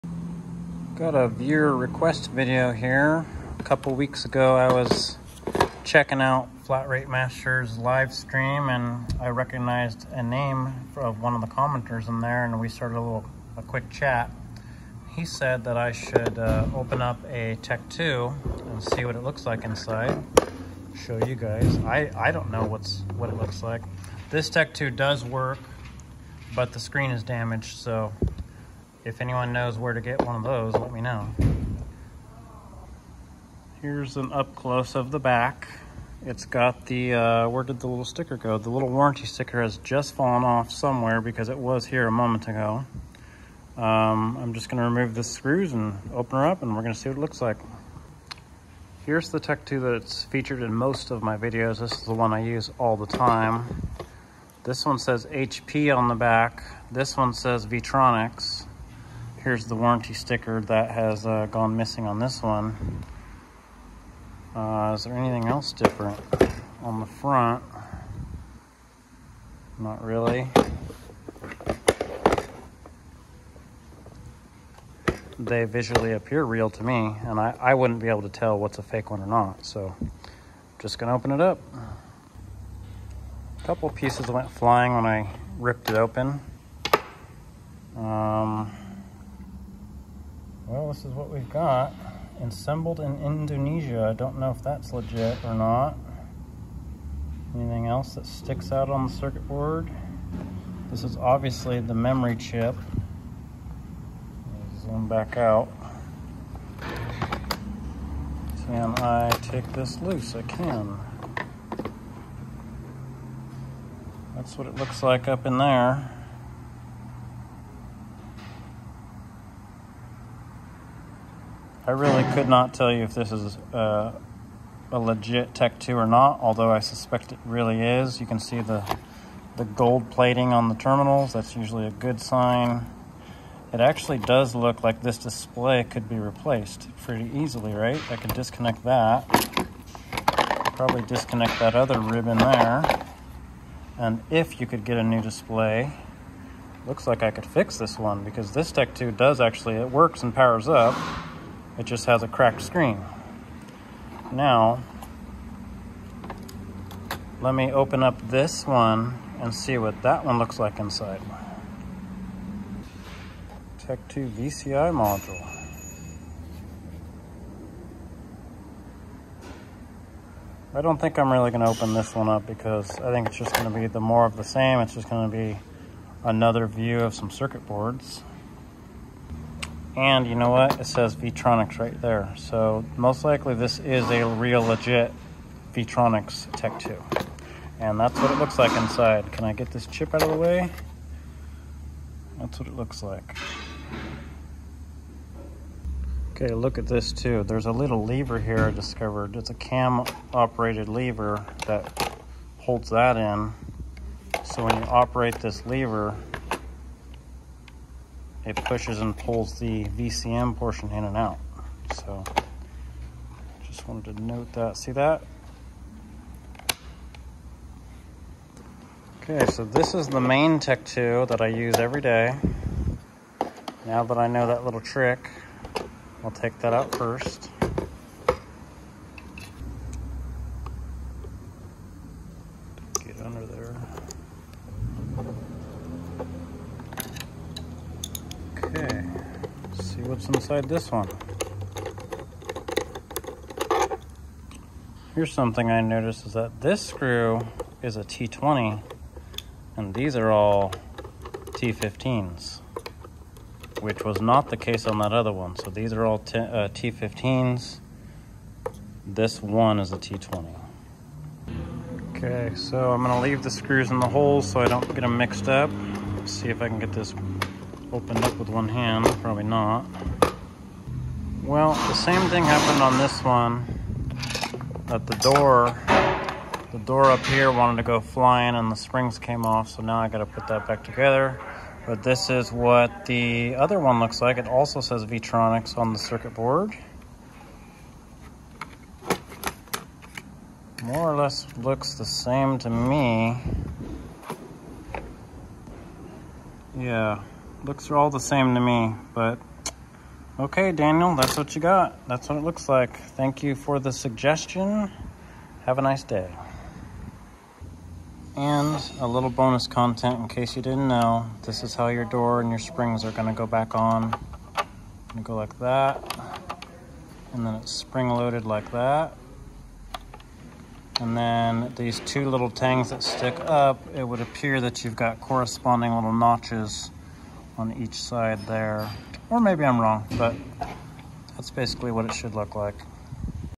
Whats inside this broken GM Tech 2 scan tool?... Viewer Request
Disassembly of GM Tech 2 to take a look at the circuit board